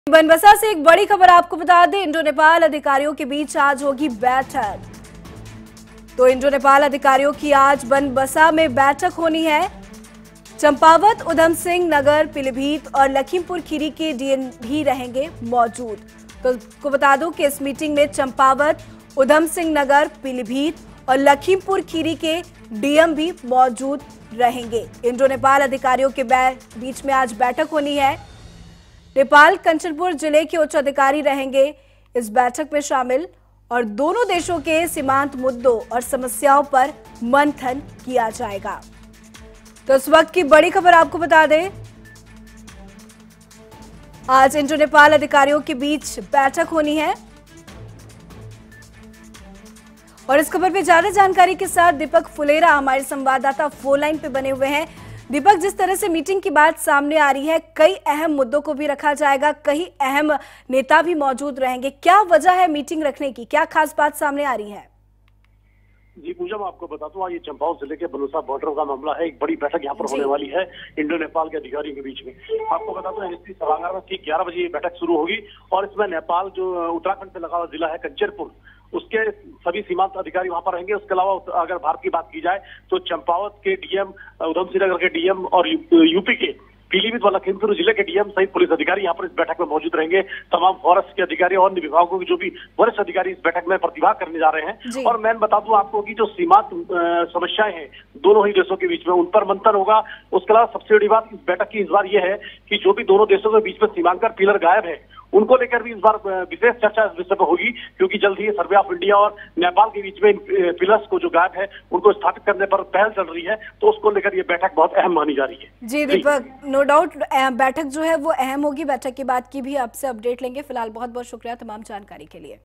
भारत-नेपाल के अधिकारियों के बीच बैठक आज
भारत नेपाल सीमा पर क्षतिग्रस्त पिलरों के सर्वेक्षण के लिए दोनों देशों के अधिकारियों के बीच होगी बैठक होगी। बैठक में नेपाल के कंचनपुर व कैलाली जिले के डीएम व एसपी और चम्पावत, पीलीभीत, यूएस नगर व लखीमपुर खीरी जिले के डीएम, एसएसपी विचार विमर्श करेंगे।